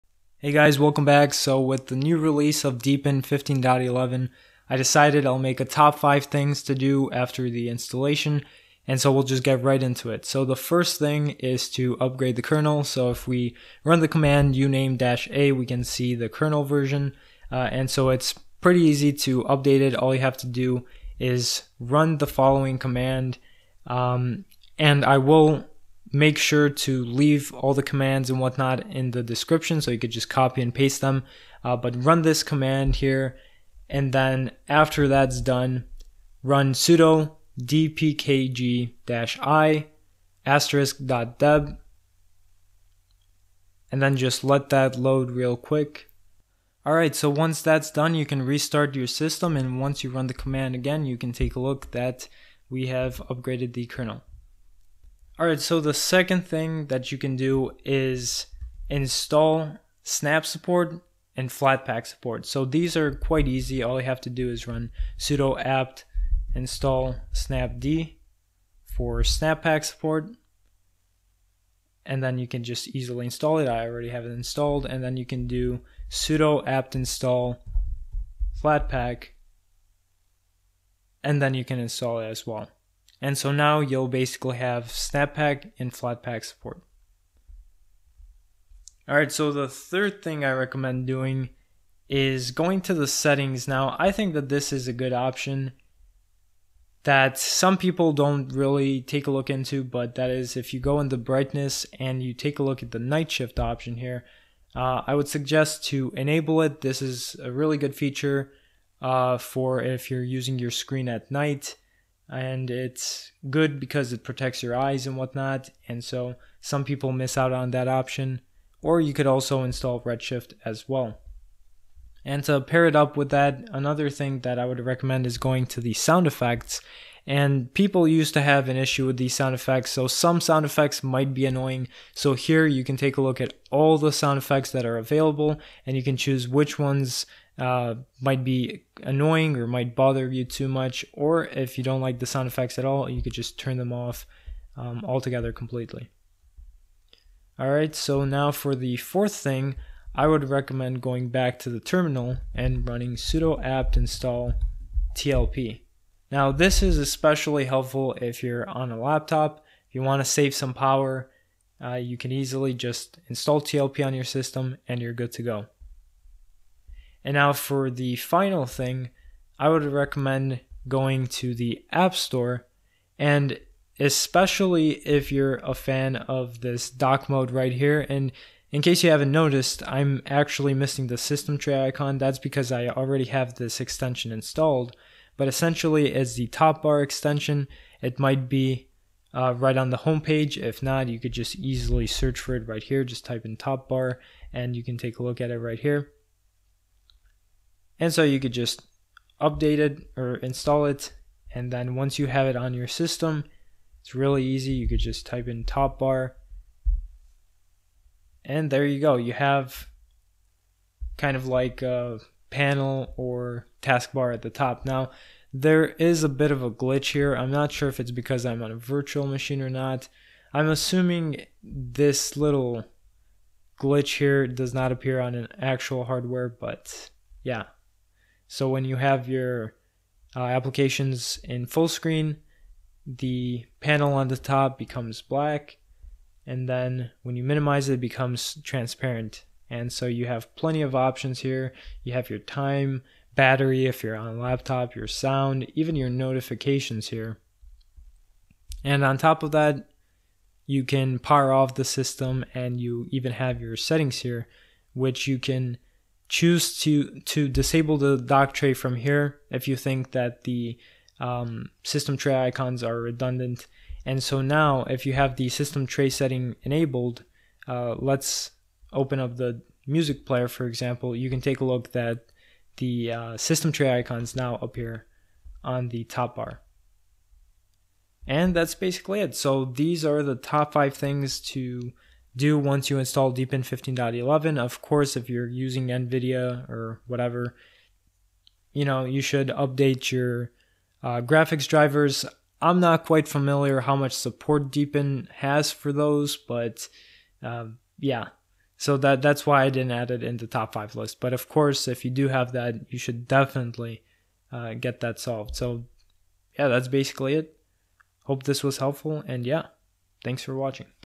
TOP 5 Things to Do After Installing Deepin 15.11
Kernel:
for the instructions

Snap/Flatpak:
sudo apt update
sudo apt install snapd
sudo apt install flatpak

TLP:
sudo apt install tlp